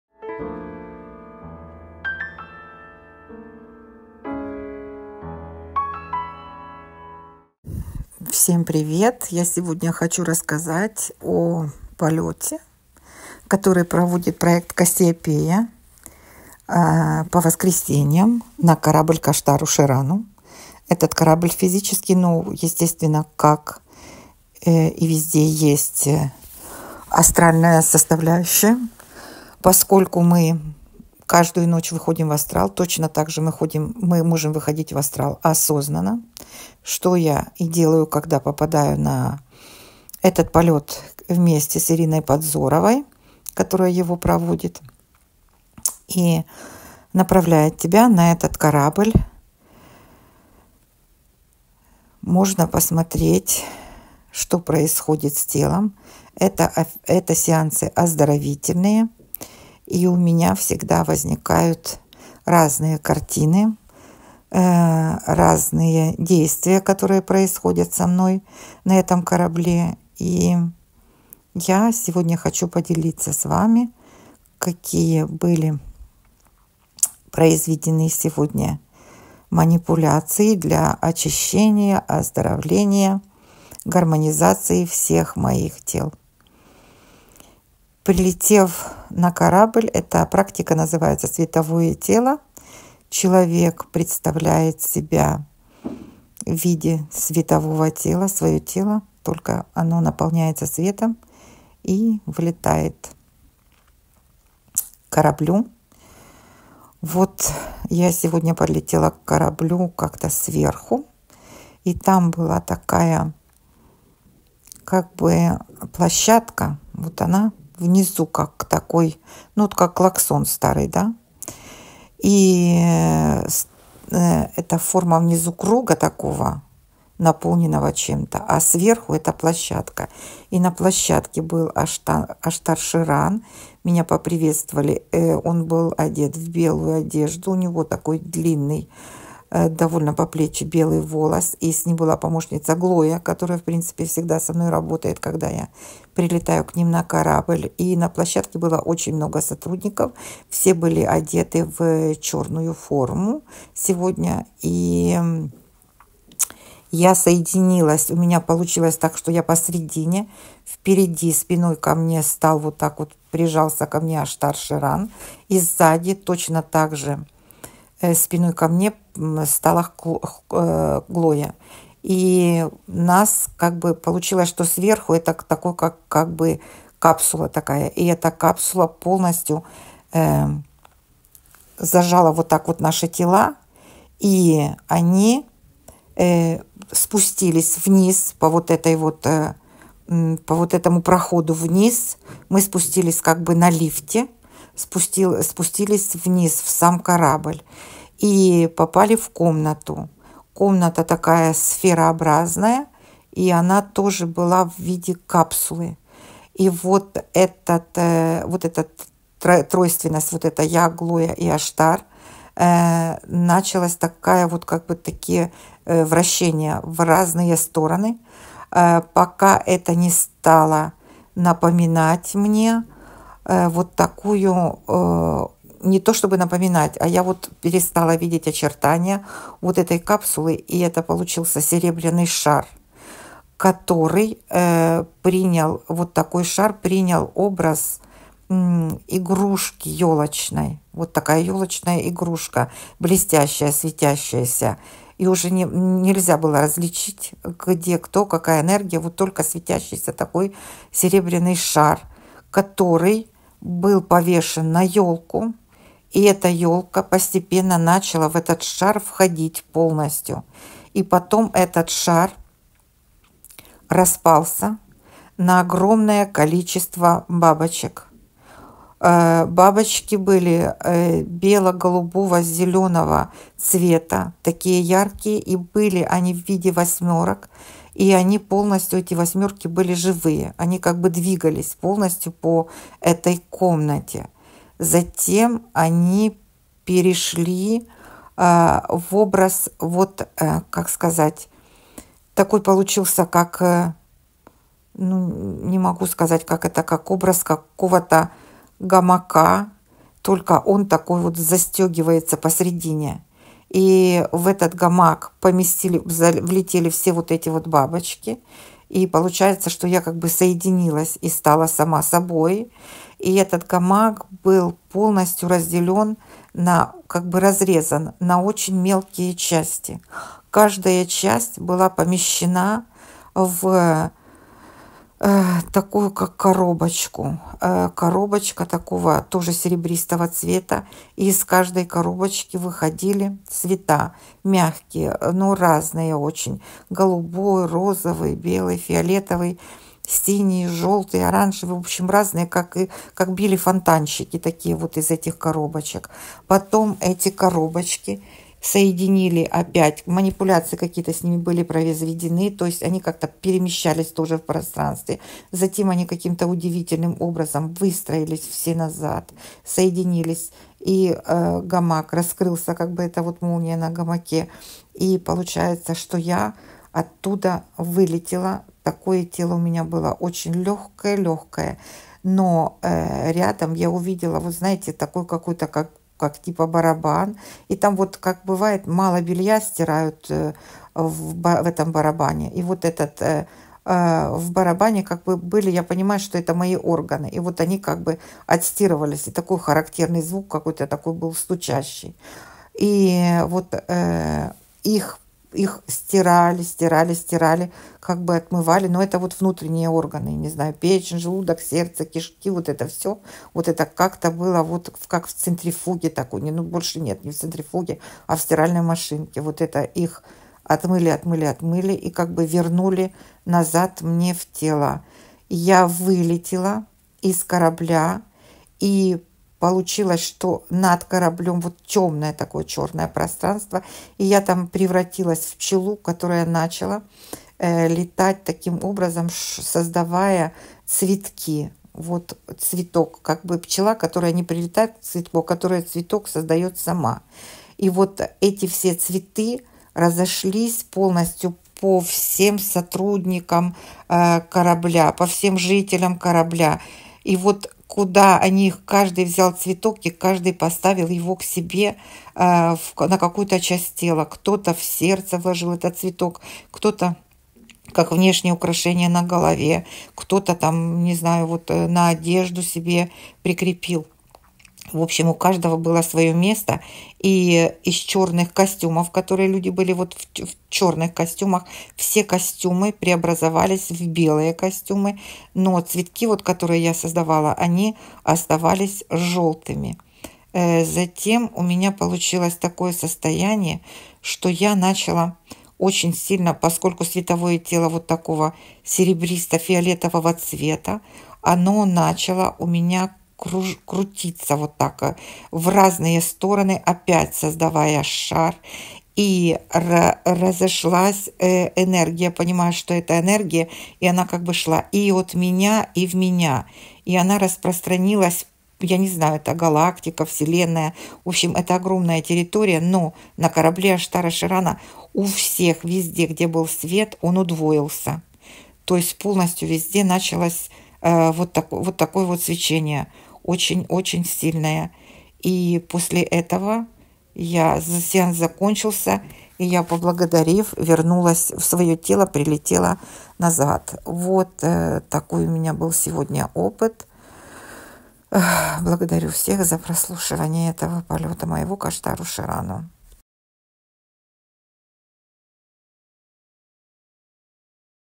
Полет к Аштару Шерану на оздоровительный сеанс — по понедельникам
Оздоровительный Полёт на корабль Аштара Шерана проводит Ирина Подзорова по понедельникам. Вход свободный ;)
на канале:
до встреч